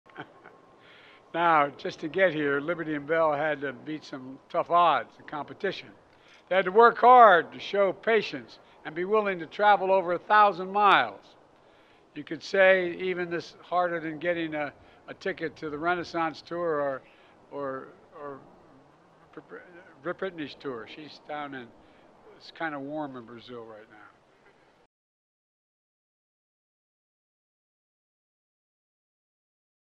Moment Joe Biden confuses Taylor Swift with 'Britney' and Beyonce | Yahoo Australia
Subscribe and turn on notifications🔔 | U.S. President Joe Biden mistakenly referred to pop superstar Taylor Swift as 'Britney' during remarks at a Thanksgiving event at the White House on Monday (November 20). Via Reuters.